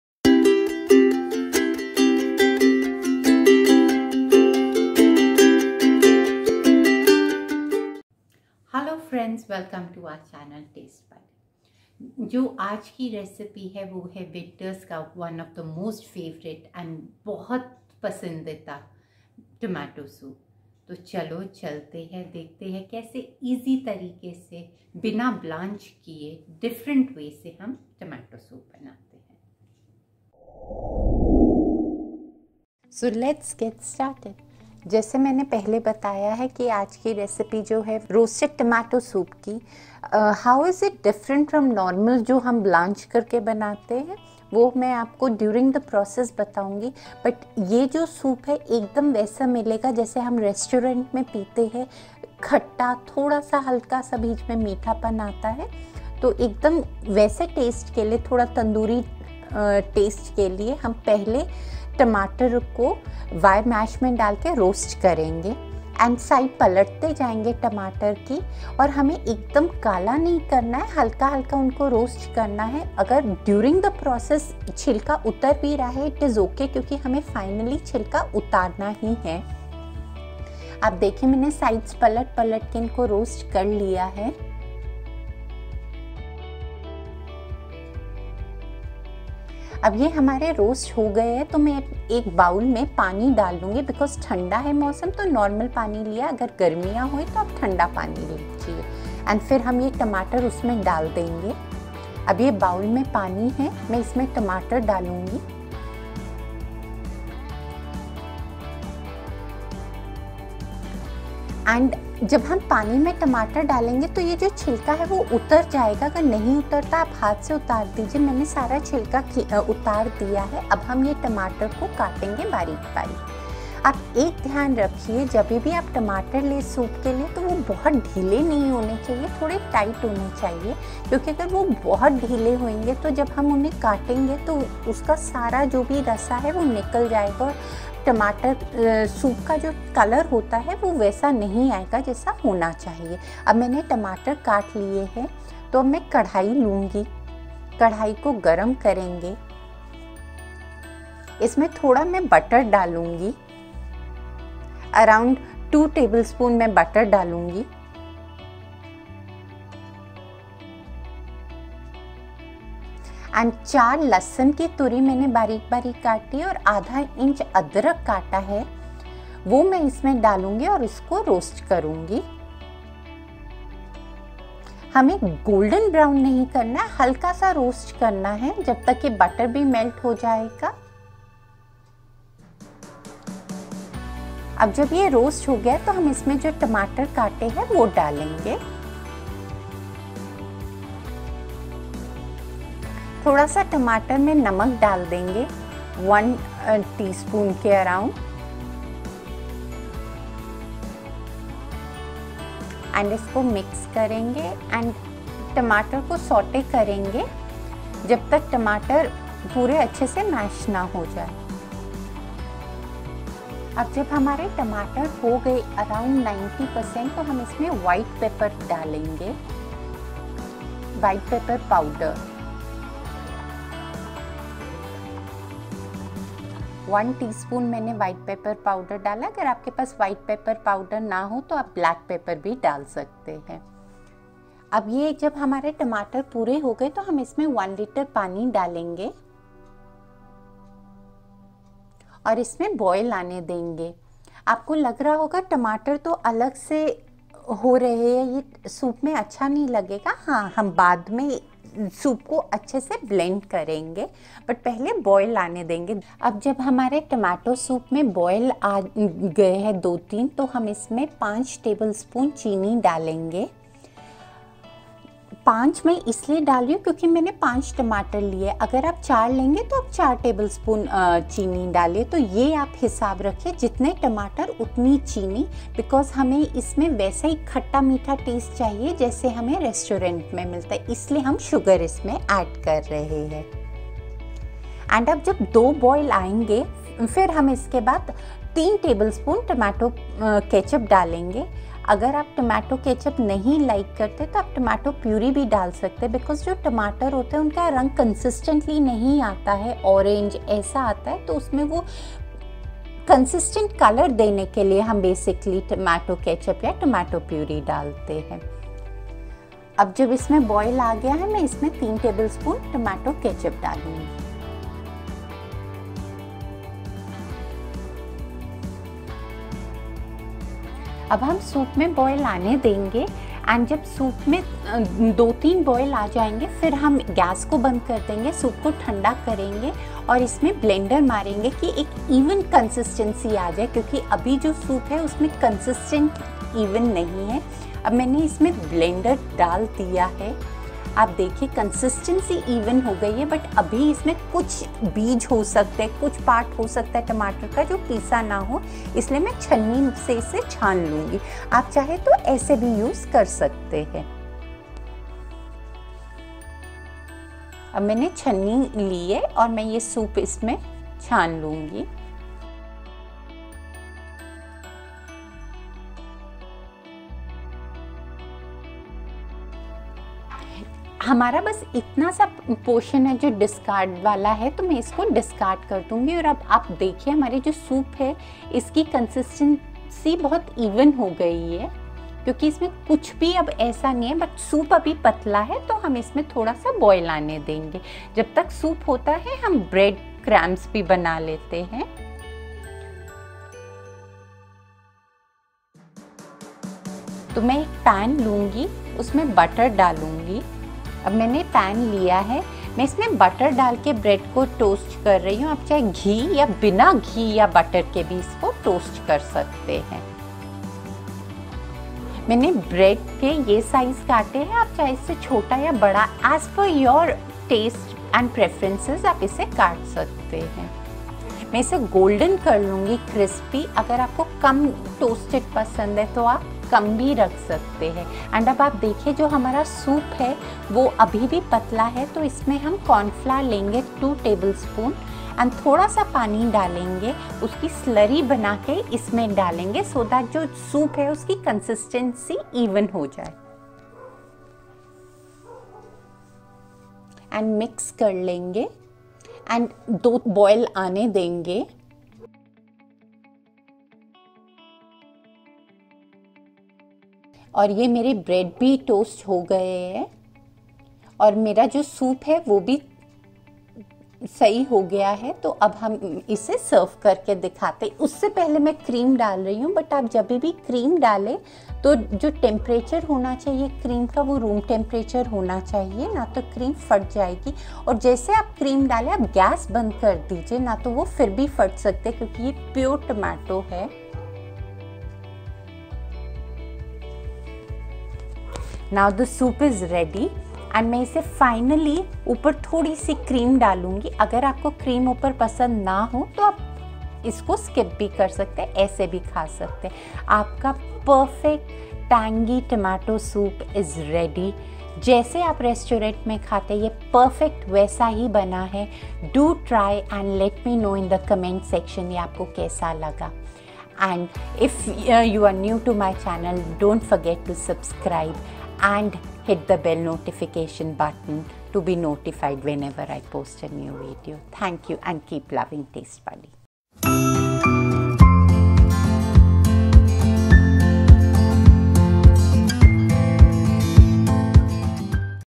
Restaurant Style Tomato Soup | रेस्टोरेंट स्टाइल टमाटर सूप | Roasted Tomato Soup
Hello All,

Taste Buddy presents Restaurant Style Tomato Soup | रेस्टोरेंट स्टाइल टमाटर सूप | Roasted Tomato Soup
Ingredients

Tomato Soup

Adrak - 1/2 inch
Lehsun - 3 Cloves
Tomato - 5
Pani - 1 L
Namak - 1 ½ tsp

Process
- Roast the Tomato on wire mash
- Add the tomatoes to the water remove the skin. Chop the tomatoes
- In a Kadhai add butter then add adrak and lehsun. Saute it for a min. Then add the chopped tomatoes.
-  Saute the Tomatoes. Add Salt. Once Tomatoes completely soften add White Pepper.
- Then Add 1 L water. Let it boil. After 2 boils. Turn off gas and blend the soup.
- Then boil soup. for 2 boils. Add corn starch.
- Let it boil for 7-10 min.
- Cut bread into small pieces. In another pan Add butter and saute the cut bread pieces.
- In soup add cream. Pour it in soup bowl. Add bread pieces. Add cream.

If you love the recipe